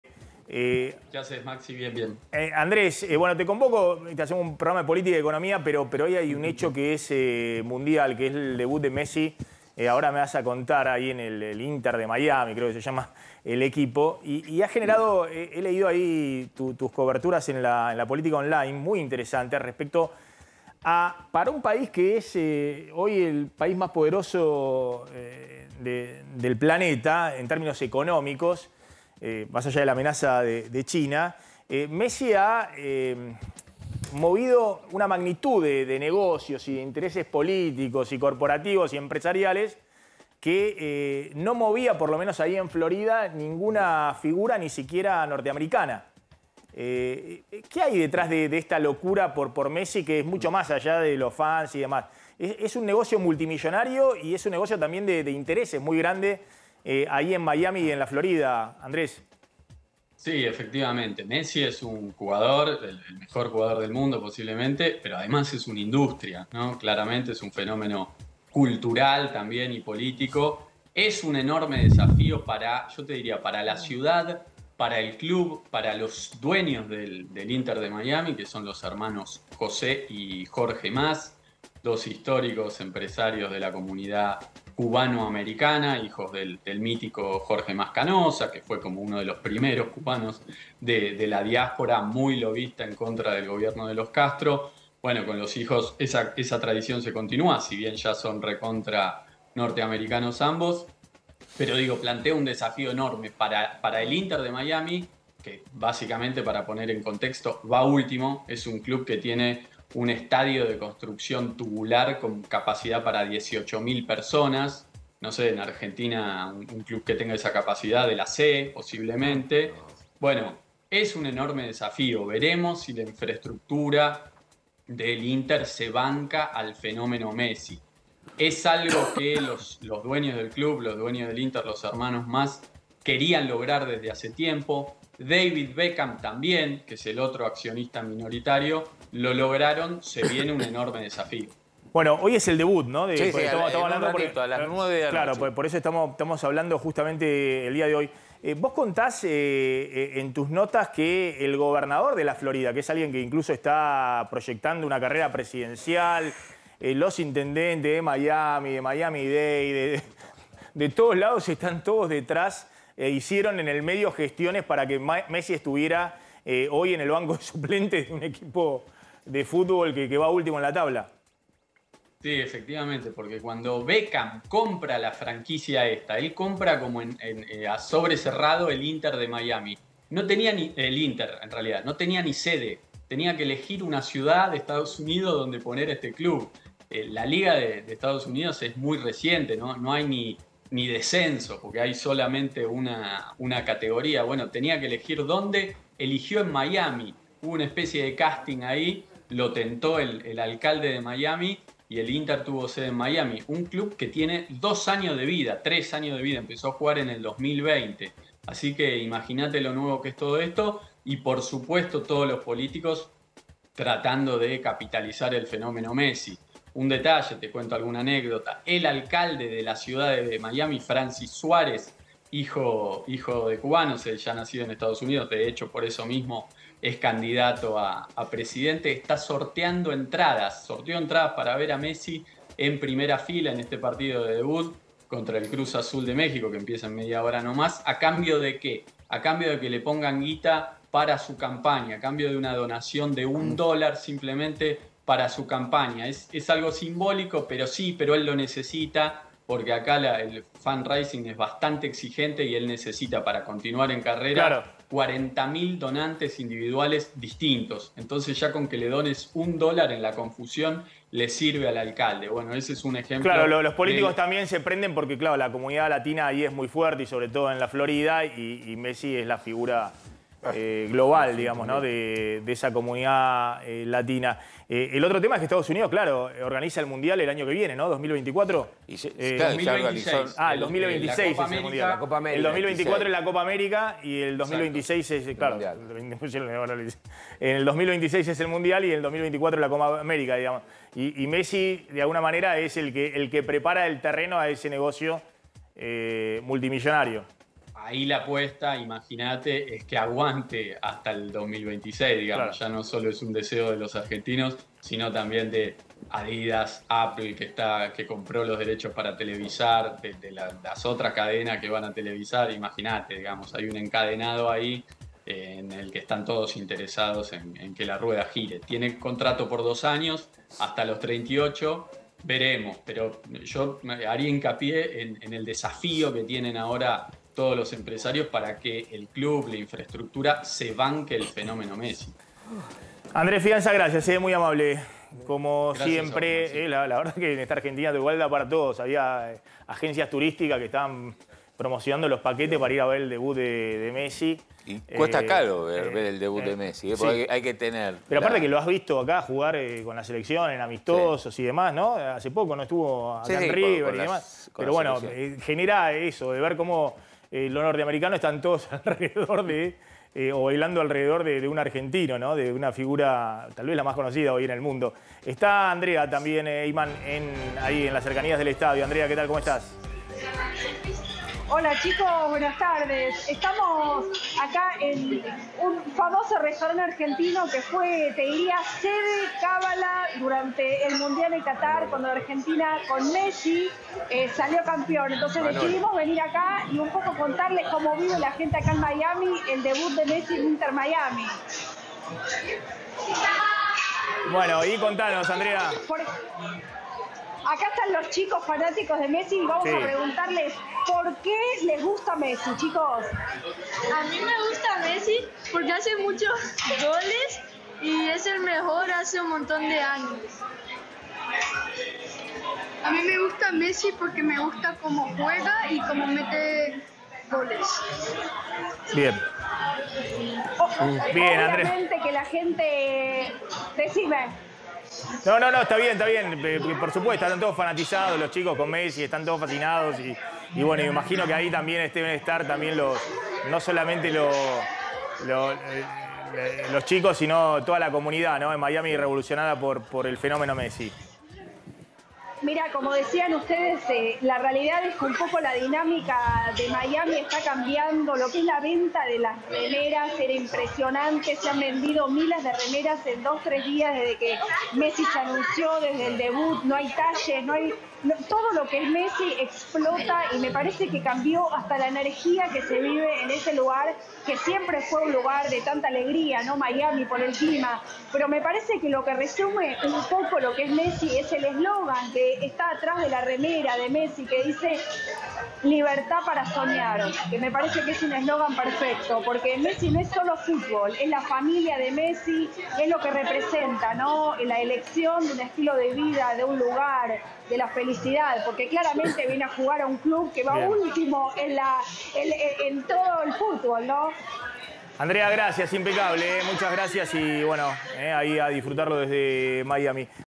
⚽ Se viene el debut de Lionel Messi en Inter Miami
¿Cuál es el negocio detrás del pase de La Pulga al club de Estados Unidos?